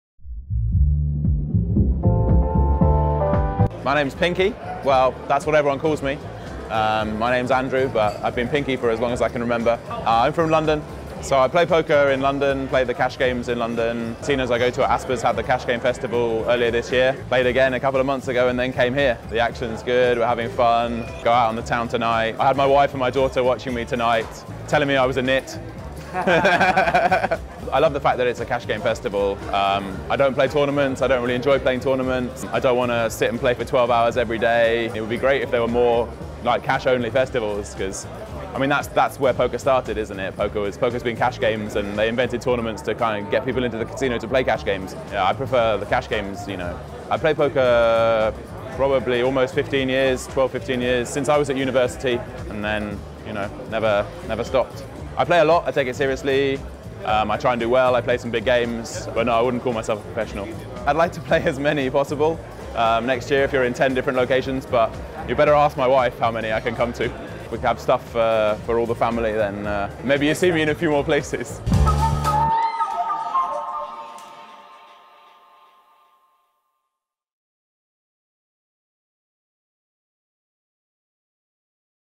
Andrew "Pinky" Seabright - Cash Game Festival Tallinn 2016
Andrew Seabright with 12-15 year experience in Poker is sharing his view of Cash Game Festival